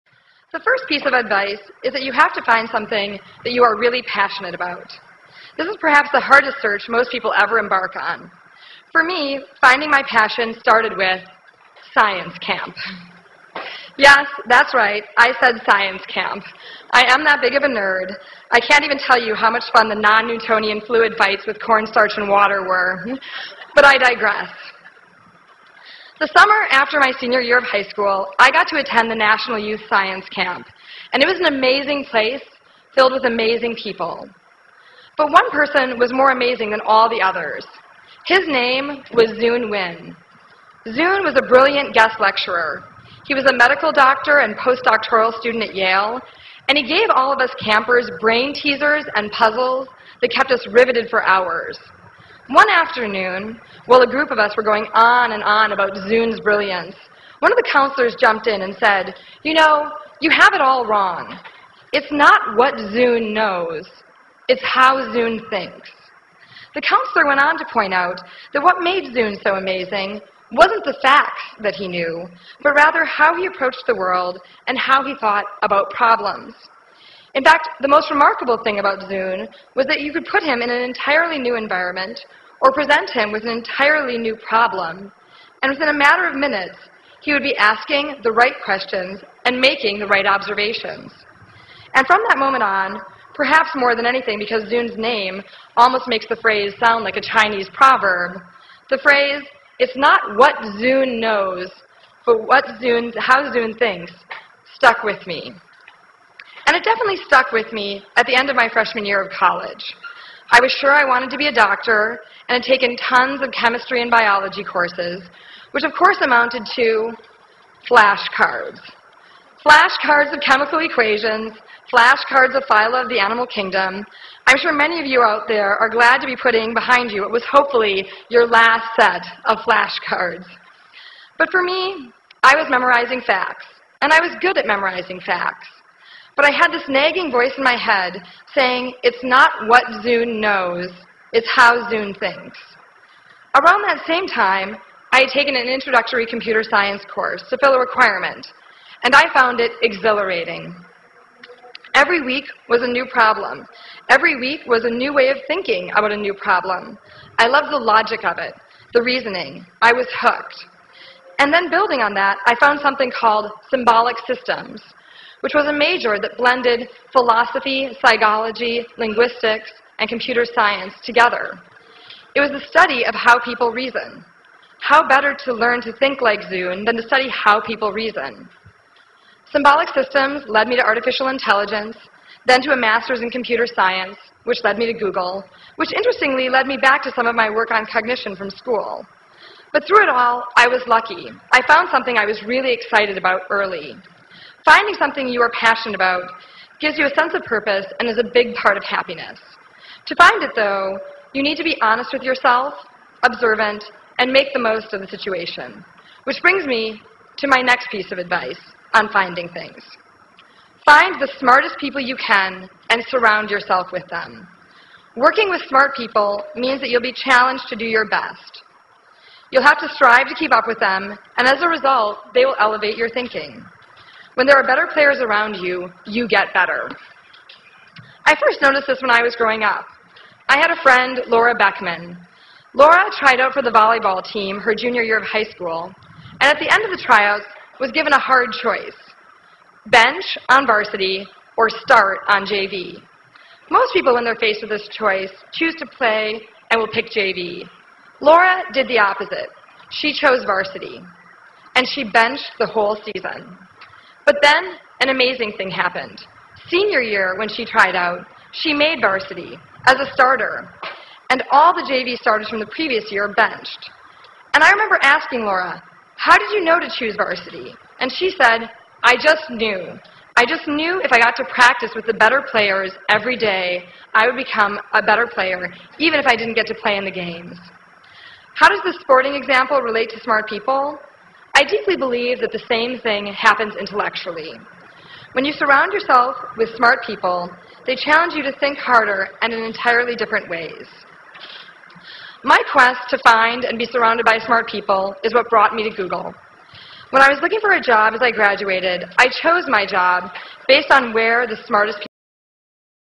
EDITED: Marissa Mayer commencement address Part 1
Commencement address of Marissa Ann Mayer at the Illinois Institute of Technology. Marissa is the VP of Search Products and User Experience of Google. She is the first woman engineer at Google and considered as one of the influential personalities in technology. She specializes in artificial intelligence and has a bachelors degree in symbolics systems and a masters in computer science from Stanford.